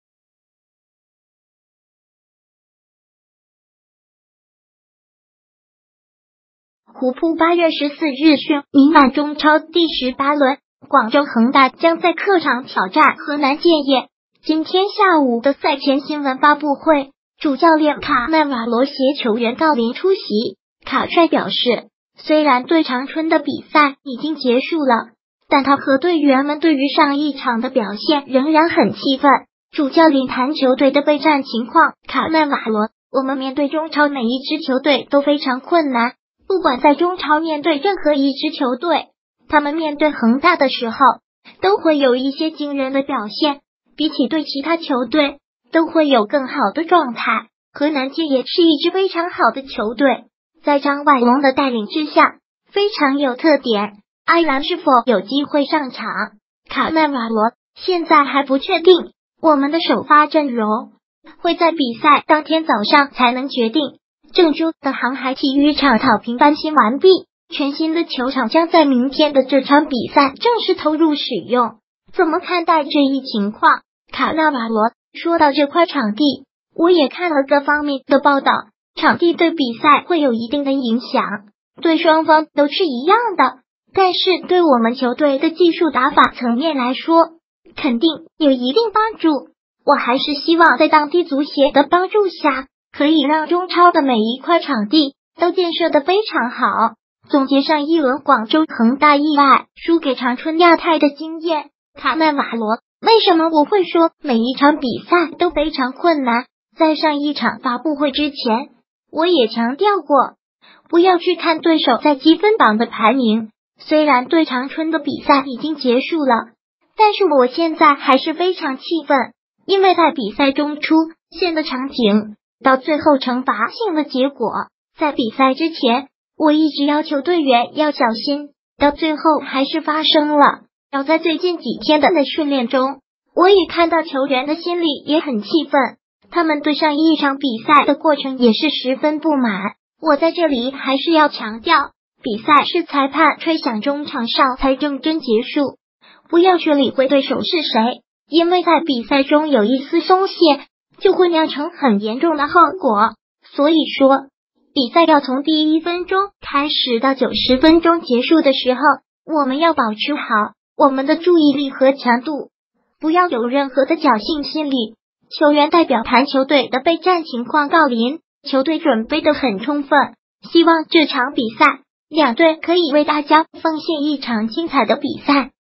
卡帅：球员们对上一轮的表现很愤怒，阿兰出场待定
虎扑8月14日讯 明晚中超第18轮，广州恒大将在客场挑战河南建业。今天下午的赛前新闻发布会，主教练卡纳瓦罗携球员郜林出席。卡帅表示，虽然对长春的比赛已经结束了，但他和队员们对于上一场的表现仍然很气愤。主教练谈球队的备战情况卡纳瓦罗：我们面对中超每一支球队都非常困难，不管在中超面对任何一支球队，他们面对恒大的时候，都会有一些惊人的表现，比起对其它球队，都会有更好的状态。河南建业是一支非常好的球队，在张外龙的带领之下，非常有特点。阿兰是否有机会上场？卡纳瓦罗：现在还不确定，我们的首发阵容，会在比赛当天早上才能决定。郑州的航海体育场草坪翻新完毕，全新的球场将在明天的这场比赛正式投入使用，怎么看待这一情况？卡纳瓦罗：说到这块场地，我也看了各方面的报道，场地对比赛会有一定的影响，对双方都是一样的，但是对我们球队的技术打法层面来说，肯定有一定帮助，我还是希望在当地足协的帮助下，可以让中超的每一块场地都建设得非常好。总结上一轮广州恒大意外输给长春亚泰的经验卡纳瓦罗：为什么我会说每一场比赛都非常困难？在上一场发布会之前，我也强调过，不要去看对手在积分榜的排名，虽然对长春的比赛已经结束了，但是我现在还是